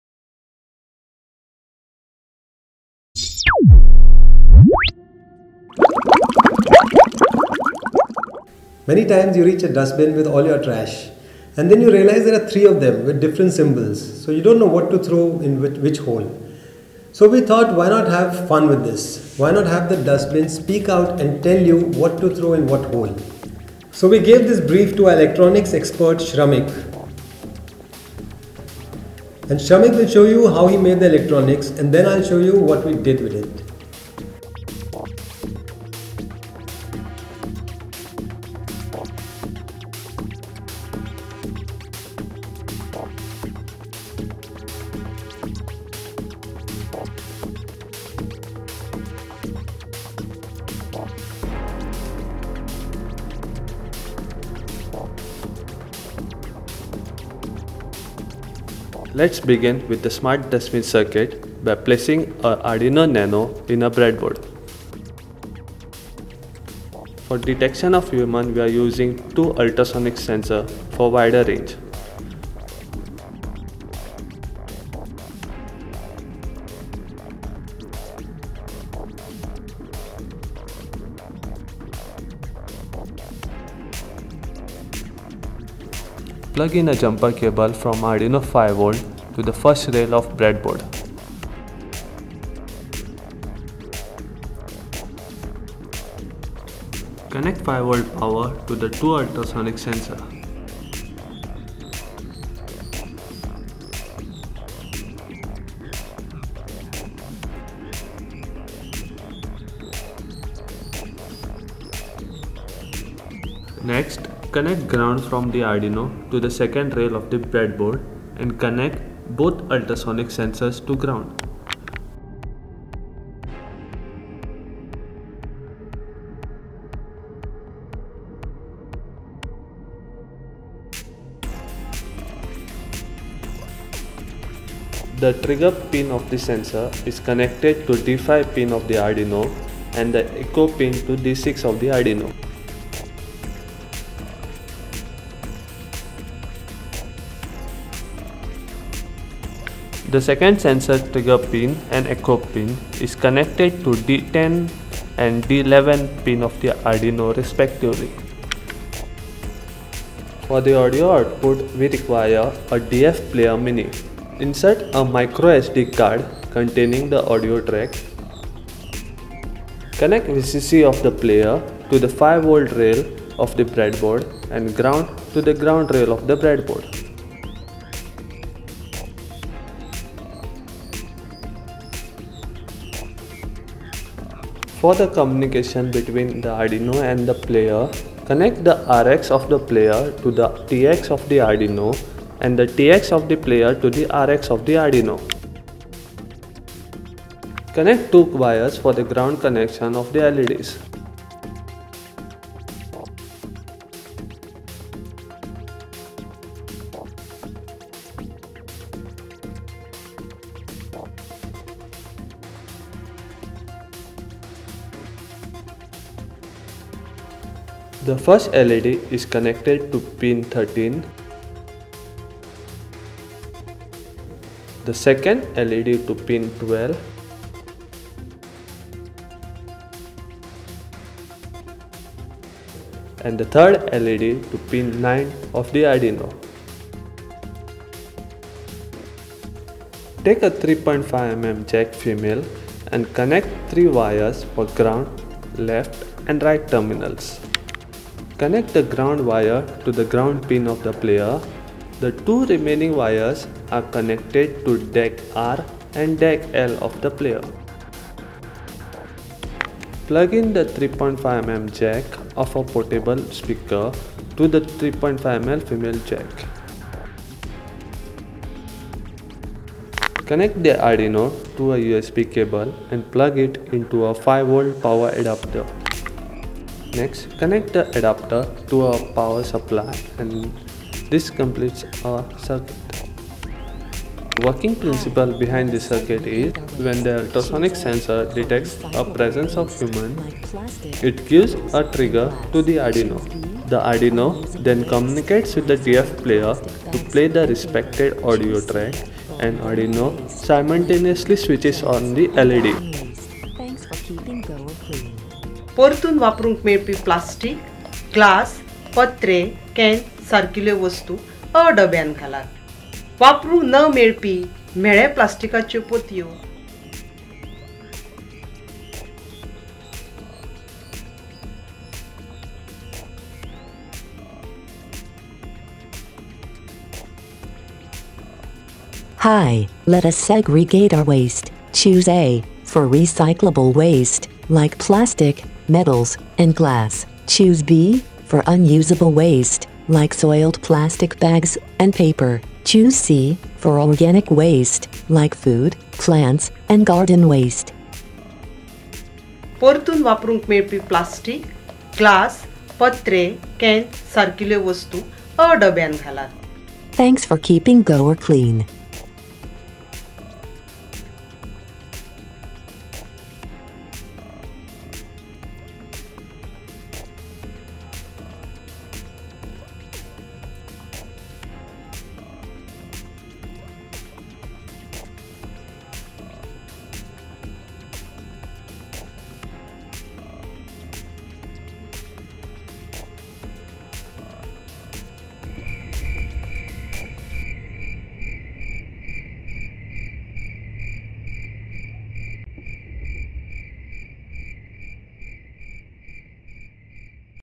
How to make Smart Dustbin with Arduino | Arduino Project | dArtofScience
Hi Guys, in this video i have shown How to make Smart Dustbin with Arduino that TALKS to you to TELL you how to SEGREGATE your WASTE. Its not a DIY but i have shown everything so that incase you want to make it, you actually can. Some amount of creativity will be required at your end which i think you will manage.

And we made the smart segregator separate from the dustbin because then you could put it in front of any 3 standard public dustbins as well.

Things Required for DIY Smart Dustbin
Speakers
3.5 mm Jack
Power Adapter
Wires
USB Cable
Arduino Nano
DF Player Mini
Micro SD Memory Card
Bread Board
HC-SR04 Ultrasonic Sensor
LED's 3 Colours